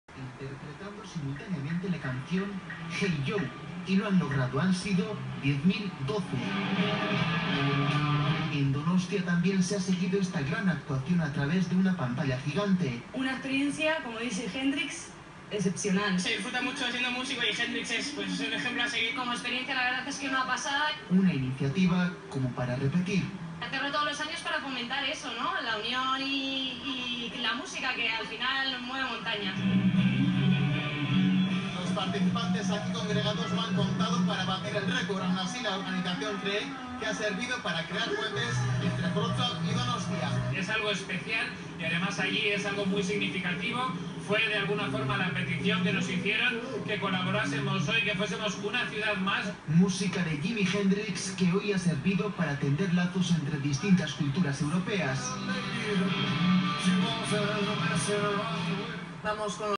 Púa de Oro, Leize Gorria, en eitb, apoyando el esfuerzo de Donostia Capital Europea, 2016.
La Escuela Biurrun, la Asoc, Leize Gorria, la Urrezko Pua, sumados al esfuerzo de dss2016. Mariana biurrun por eitb, la tv vasca.